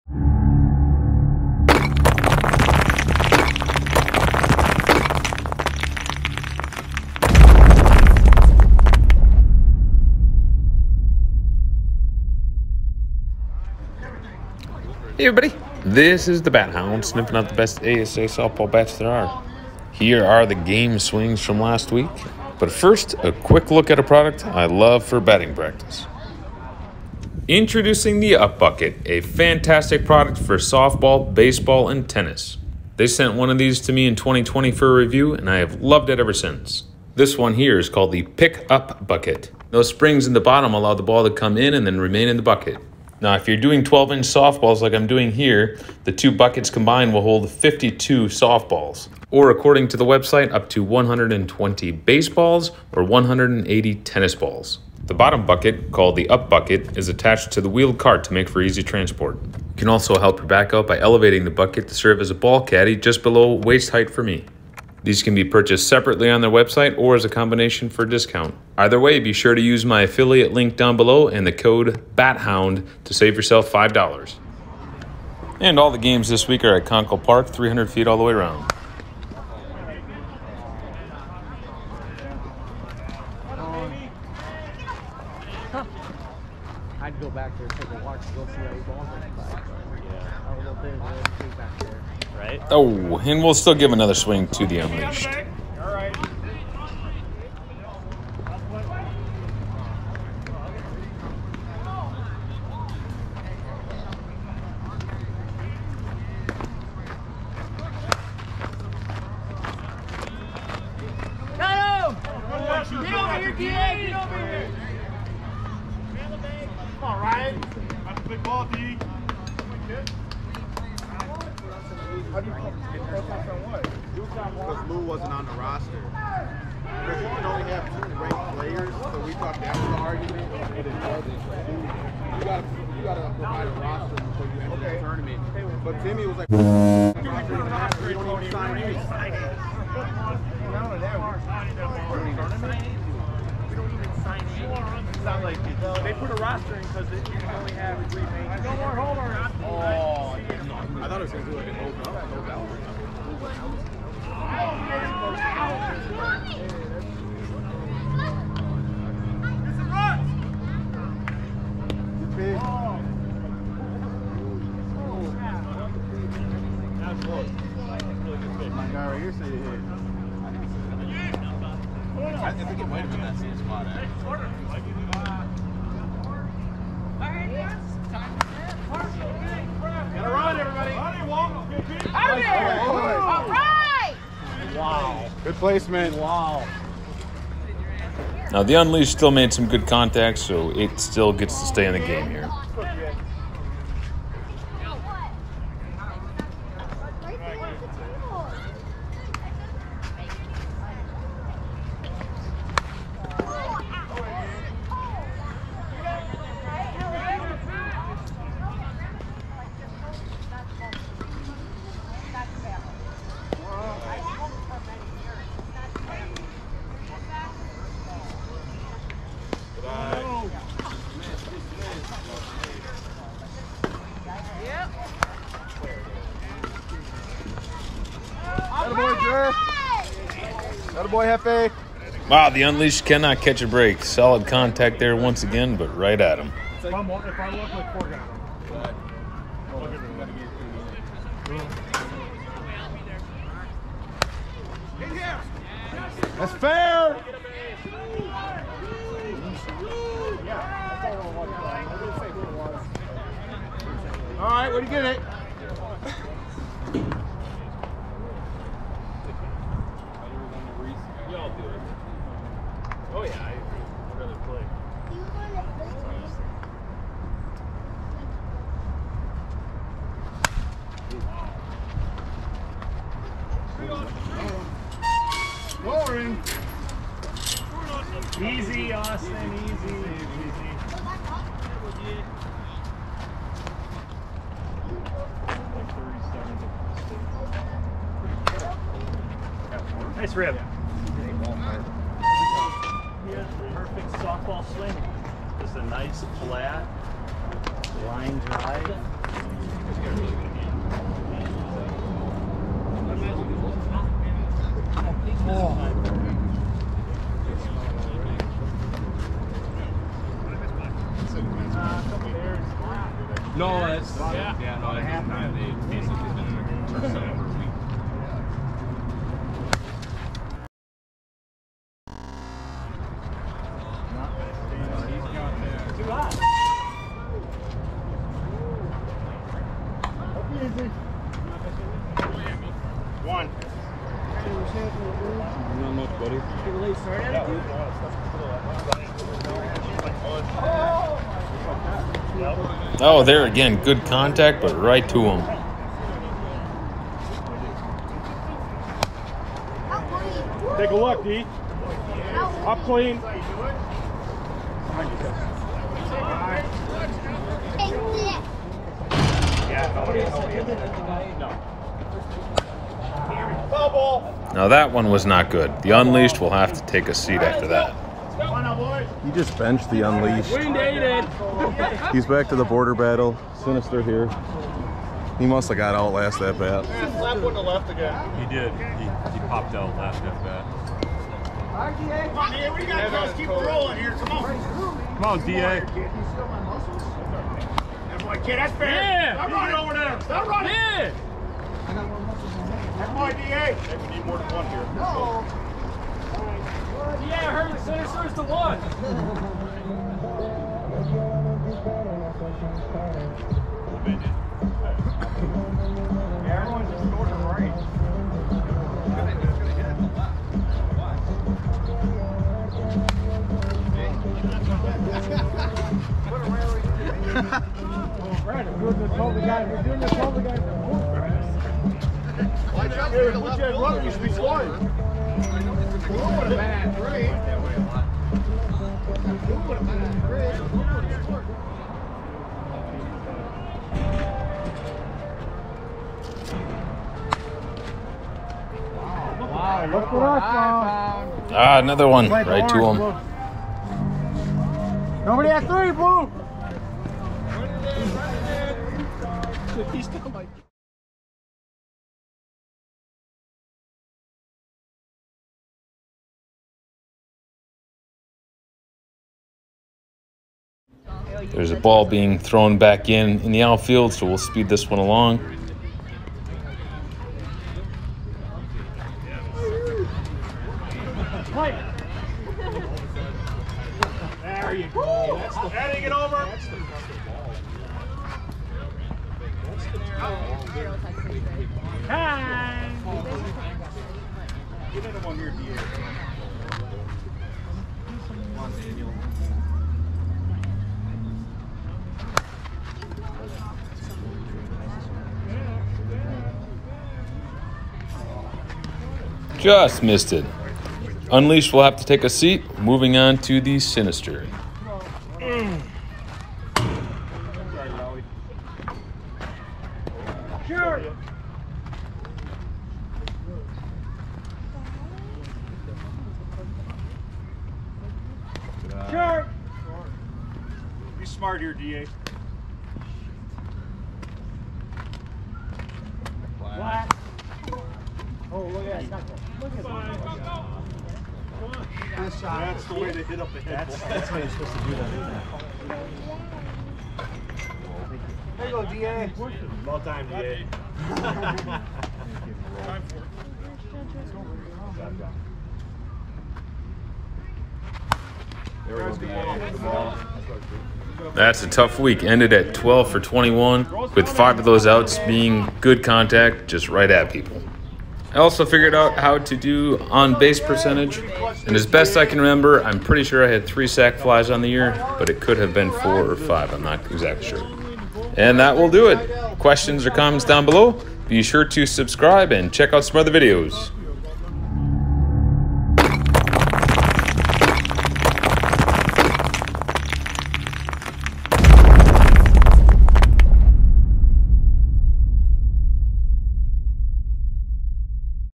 GAME SWINGS #14 (2021)
Here are the captured game swings from last week. The Monsta Unleashed continues as the gamer!